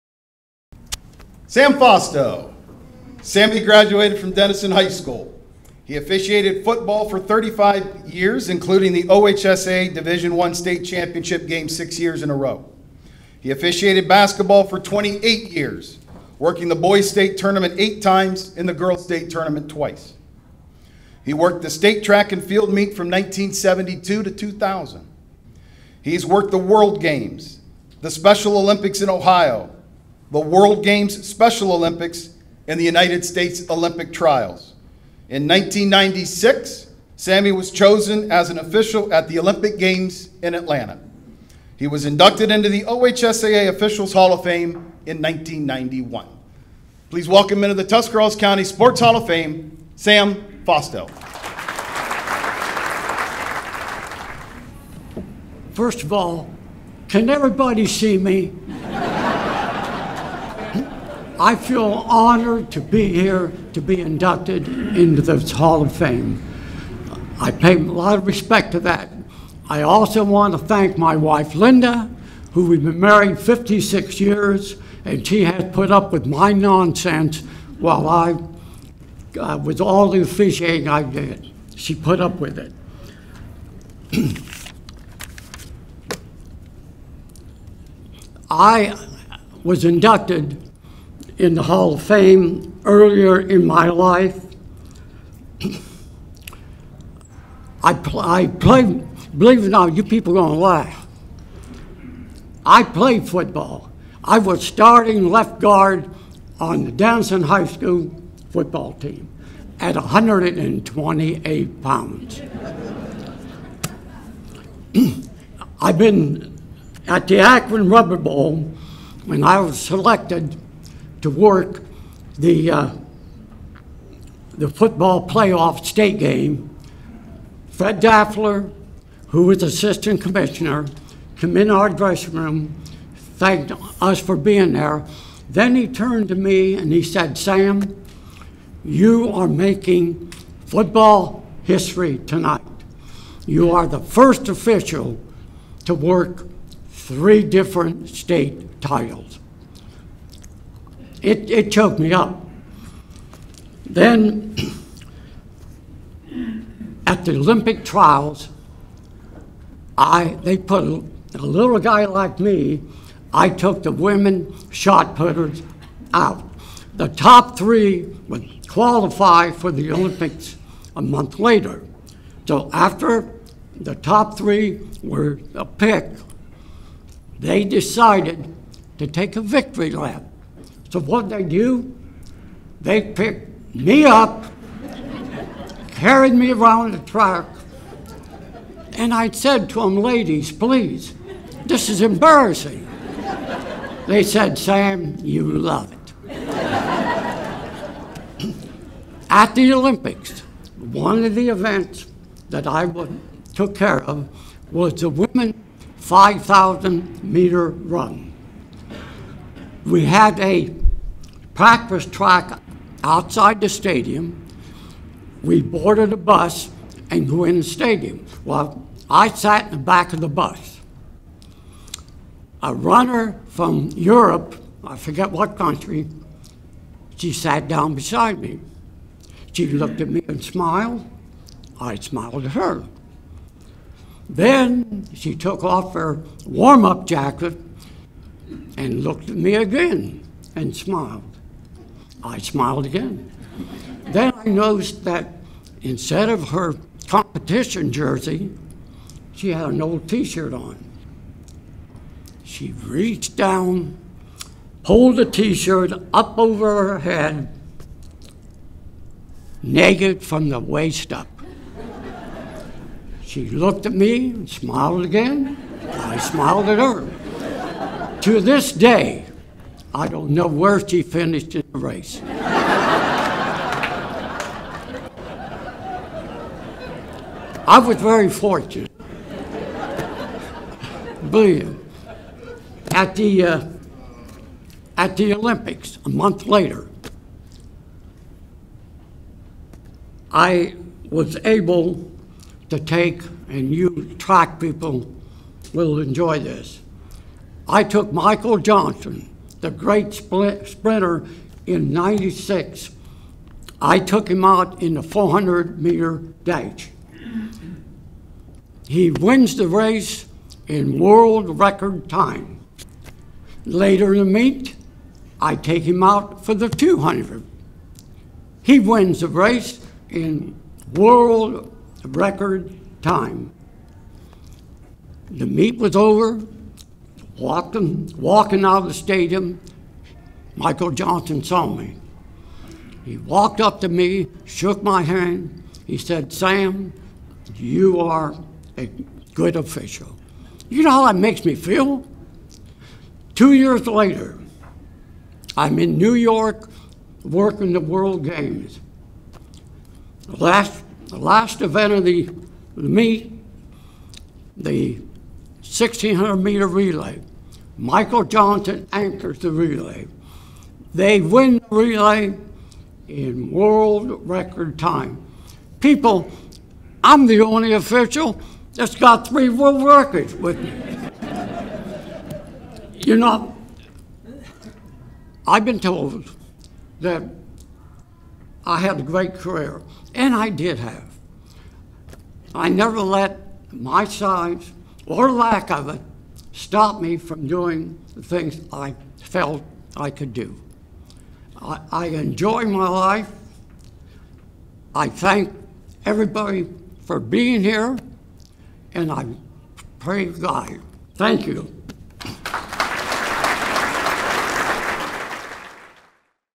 Sam Fausto Hall of Fame Induction Speech
In this video Sam Fausto delivers his acceptance speech as part of the inaugural class of the Tuscarawas County Sports Hall of Fame.  This video was recorded on Saturday, June 18, 2022 and was filmed at Buckeye Career Center in New Philadelphia, Ohio.